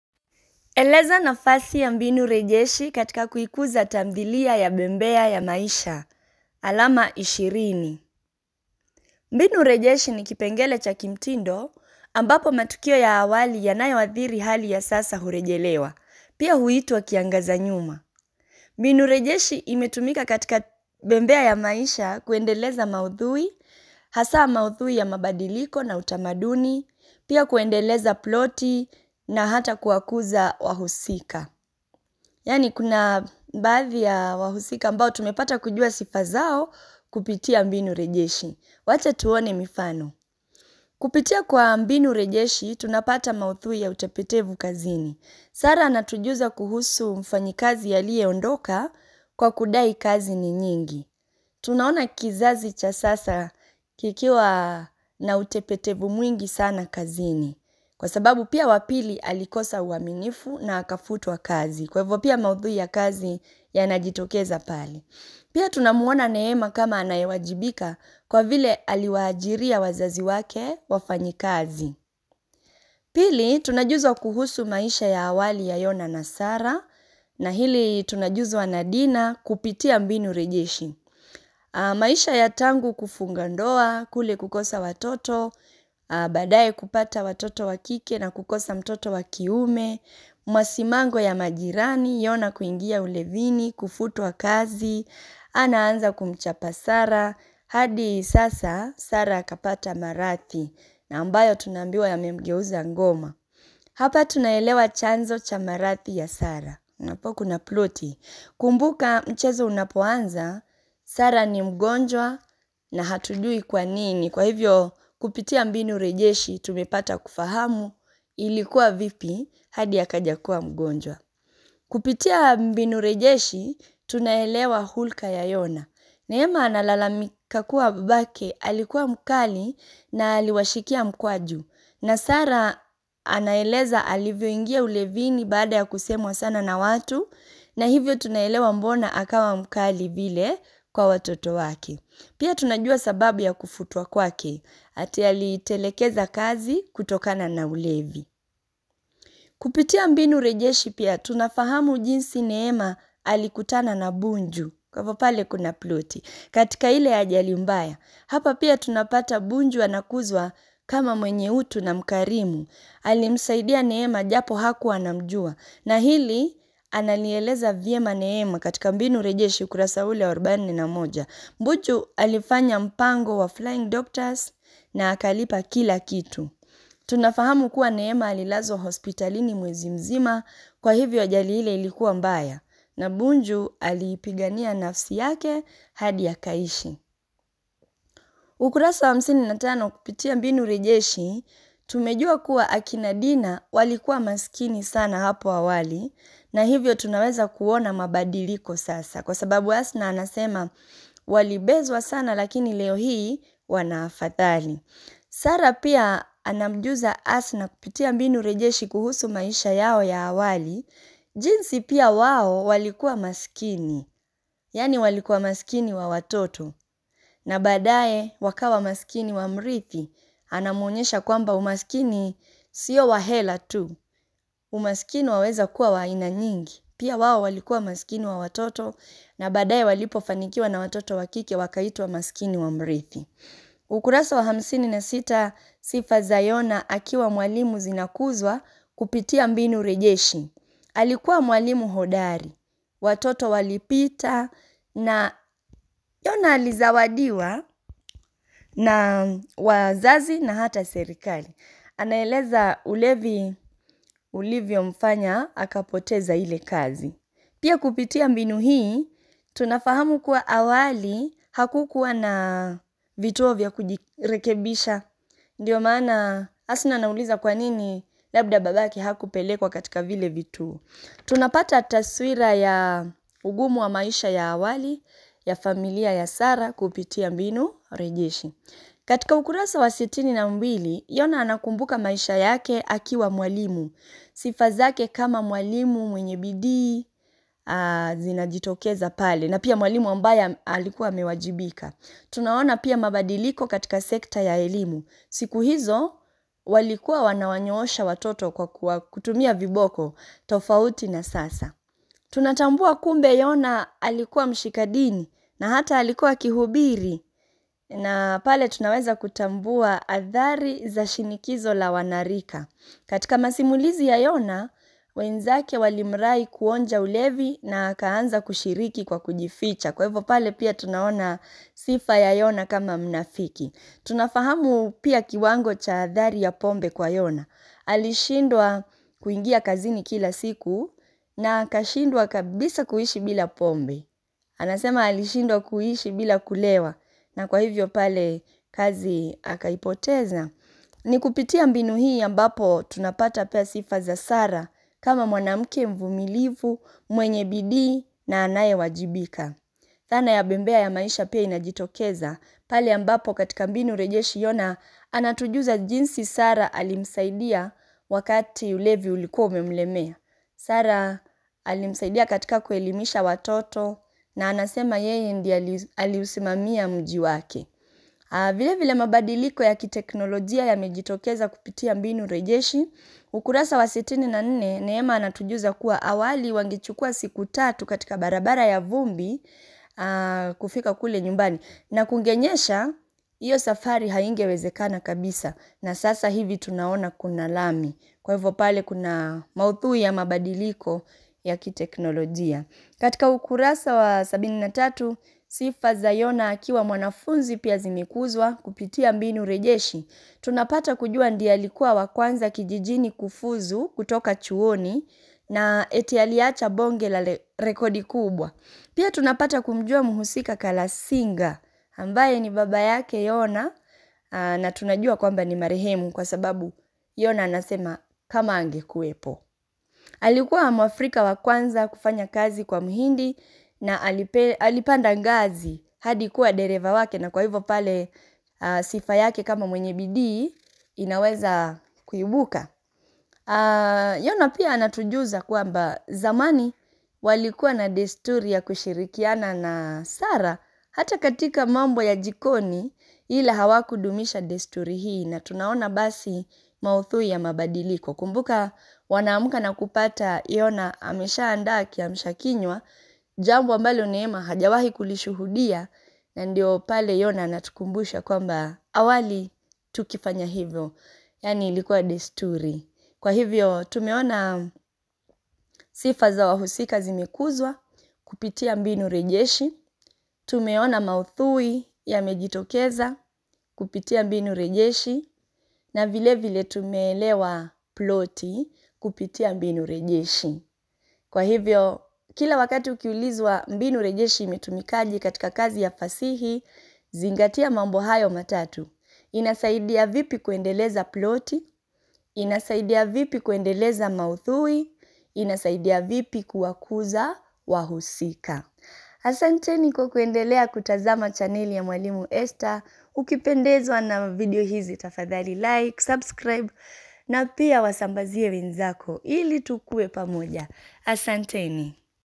Maswali ya Bembea ya Maisha/Mbinu rejeshi katika Bembea ya Maisha/ mwongozo wa Bembea ya Maisha pdf/
Matumizi ya mbinu rejeshi katika kuendeleza ploti, maudhui na wahusika.